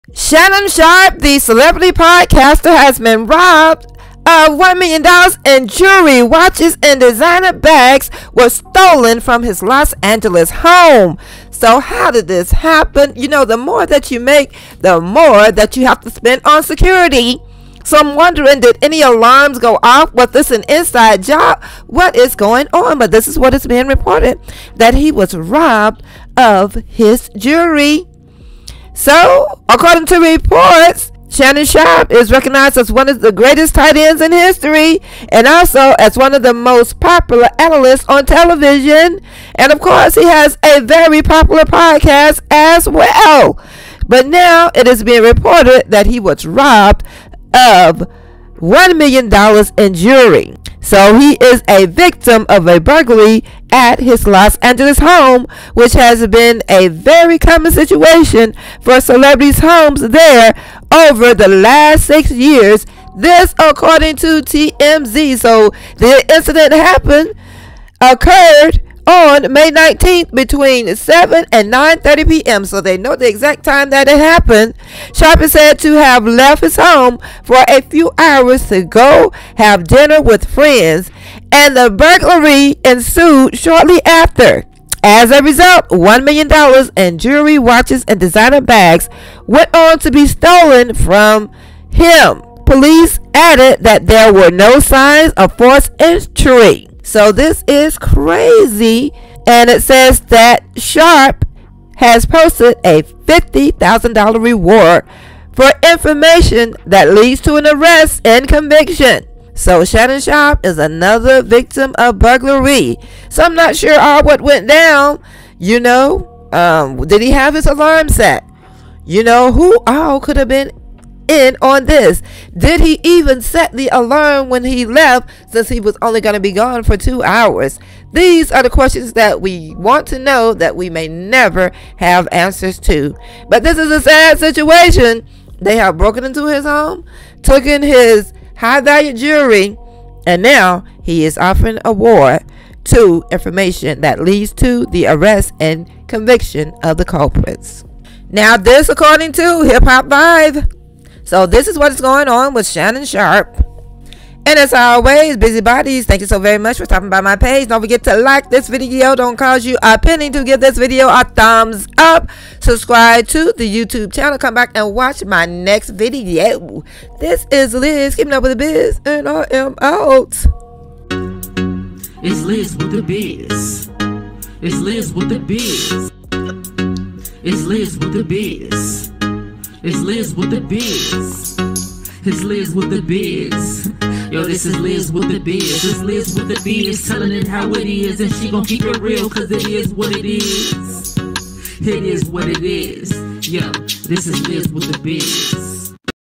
Shannon Sharpe Million Dollars in Jewelry Taken From Celebrity Bloggers Home
One Million Dollar in Jewelry taken from shannonsharpe LA Home

LizWitthaBiz, KeepingUpWithTheBiz, KeepingUpWitThaBiz, Keeping Up with The Biz, Liz With The Biz, It's Liz With The Biz, Its Liz With The Biz